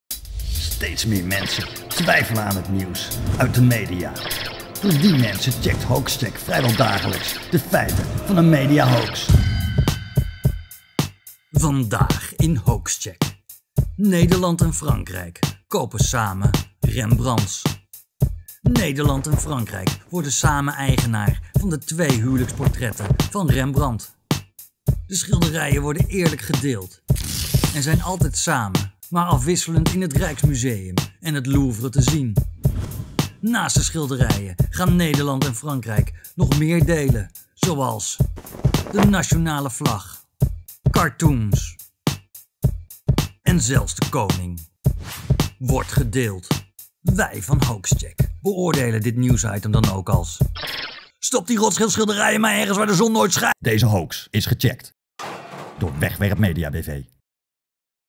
HoaxCheck: Nederland en Frankrijk kopen samen portretten Rembrandt
Nederland en Frankrijk zijn tot een akkoord gekomen over de aankoop van de twee familieportretten van Rembrandt.

Dat meldt minister Jet Bussemaker van Cultuur woensdag aan de Tweede Kamer.

"Nu ook aan Franse zijde onmiddellijk financiering beschikbaar is, is het kabinet voornemens de huwelijksportretten gezamenlijk met Frankrijk te verwerven", aldus Bussemaker.

Dat betekent dat de kunstwerken niet geheel in Nederlandse handen komen, zoals het kabinet en het Rijksmuseum dat graag hadden gezien.
160 miljoen

Begin vorige week werd bekend dat het kabinet en het Rijksmuseum in Amsterdam dicht bij de aankoop van de twee beroemde Rembrandts waren voor een totaalbedrag van 160 miljoen euro.

Het kabinet zou 80 miljoen opzij leggen voor de familieportretten van Maerten Soolmans en Oopjen Coppit uit 1634, terwijl het Rijksmuseum via private partijen de overige 80 miljoen bij elkaar moest zien te krijgen.

Na de bekendmaking van de aankoopplannen bleek ook Frankrijk een oogje te hebben op de familieportretten die door de Franse bankiersfamilie Rothschild in de verkoop waren gezet.

Louvre

Nederland en Frankrijk kopen de schilderijen nu samen. Beide landen leggen 80 miljoen euro neer. De portretten zullen, altijd in elkaars gezelschap, beurtelings in het Rijksmuseum en het Louvre te zien zijn.

"De bijzondere huwelijksportretten worden en blijven altijd samen zichtbaar in het Europese publieke domein, afwisselend in twee van de meest gerenommeerde en drukbezochte musea van Europa, het Rijksmuseum en het Louvre", schrijft de bewindsvrouw.

''We zijn bijzonder verheugd dat beide fantastische portretten nu publiek bezit worden en voorgoed bij elkaar blijven met afwisselende presentaties in het Rijksmuseum en het Louvre'', laat het Rijksmuseum in een officiële reactie weten.
Tweede Kamer

Woensdag gaf de Tweede Kamer het kabinet groen licht voor de aankoop van twee schilderijen van Rembrandt van Rijn, al wist het toen niet of het om een of twee schilderijen ging. Het kabinet liet toen al doorschemeren dat er een kans bestond dat de schilderijen samen met de Fransen zouden worden aangeschaft.

Volgens Bussemaker is gaandeweg de mogelijkheid bekeken of de kunstwerken samen met de Fransen zouden worden gekocht. Volgens de Fransen had Bussemaker daar afspraken over gemaakt, waar zij de minister aan hebben gehouden. De Fransen voelden zich geschoffeerd dat Nederland opeens bekendmaakte beide schilderijen te kopen.

Het kabinet verdedigt het mede-eigenaarschap nu door te stellen dat de schilderijen in ieder geval niet naar China worden verscheept, of "ergens op de slaapkamer van een rijke oliesjeik" komen te hangen.